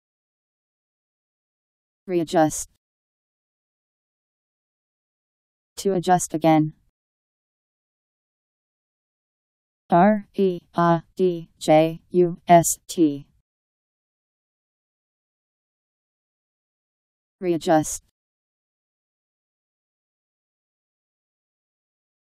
Readjust Meaning
Video shows what readjust means. to adjust again.  Readjust Meaning. How to pronounce, definition audio dictionary. How to say readjust. Powered by MaryTTS, Wiktionary